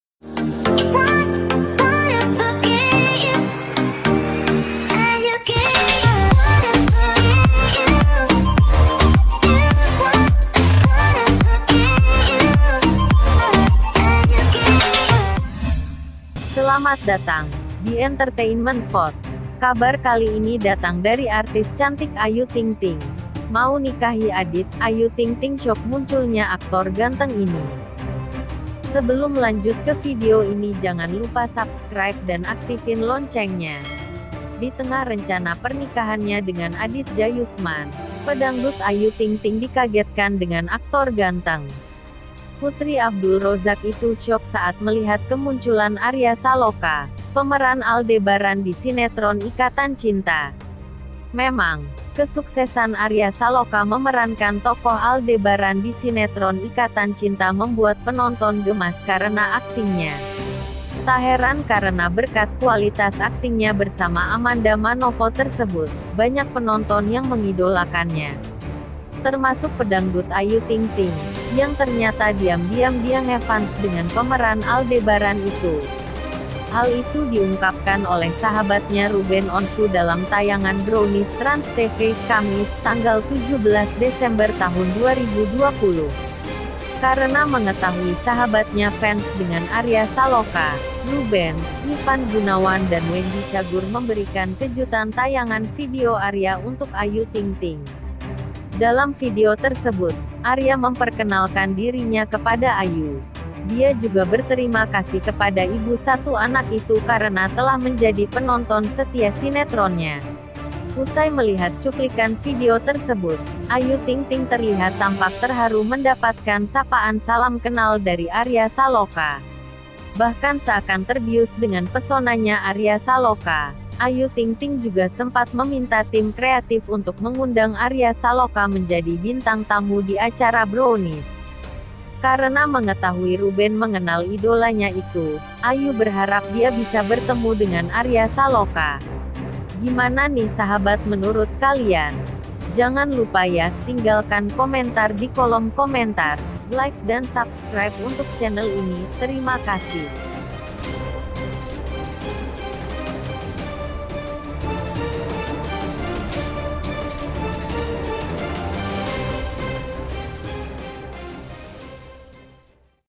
MAU NIKAHI ADIT, AYU TING TING SYOK MUNCULNYA AKTOR GANTENG INI
MAU NIKAHI ADIT, AYU TING TING SYOK MUNCULNYA AKTOR GANTENG INI Di tengah rencana pernikahannya dengan Adit Jayusman, pedangdut Ayu Ting Ting dikagetkan dengan aktor ganteng. Putri Abdul Rozak itu syok saat melihat kemunculan Arya Saloka, pemeran Aldebaran di Sinetron Ikatan Cinta. Memang, kesuksesan Arya Saloka memerankan tokoh Aldebaran di Sinetron Ikatan Cinta membuat penonton gemas karena aktingnya. Tak heran karena berkat kualitas aktingnya bersama Amanda Manopo tersebut, banyak penonton yang mengidolakannya. Termasuk pedangdut Ayu Ting Ting, yang ternyata diam-diam dia ngefans dengan pemeran Aldebaran itu.